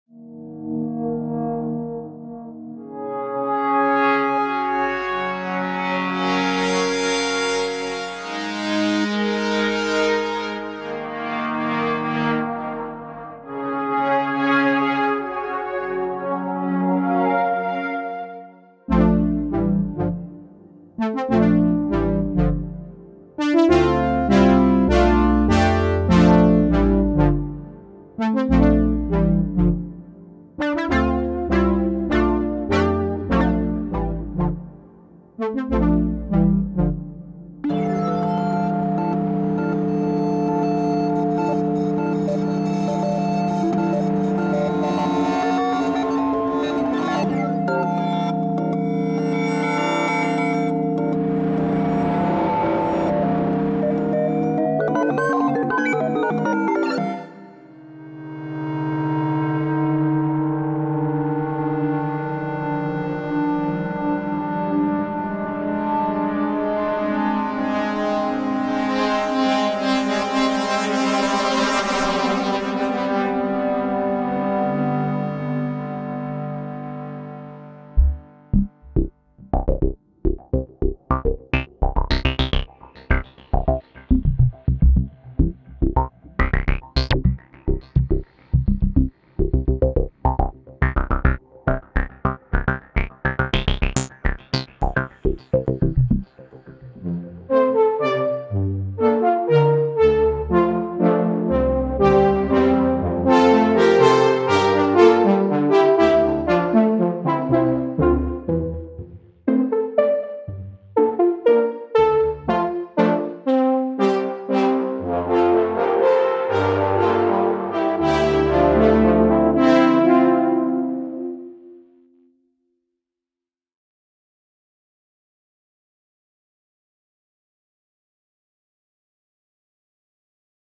Perfect Drift – a new Sound Package for Bitwig Studio
Nature doesn't draw straight lines or make static noises — and neither do the presets in the Perfect Drift sound package. These synth patches embrace asymmetries and inconsistencies to approximate analog's imperfect beauty.
This sound package contains more than 100 presets and clips of familiar patches and classic-sounding synths, some of which were modeled on the work of analog pioneers.

Examples:
00:00 – Wheel of Fire
00:19 – PlanetSynth
00:38 – Chance of Hail
00:58 – In Memorium
01:18 – Moogy Gates
01:37 – Silent Brass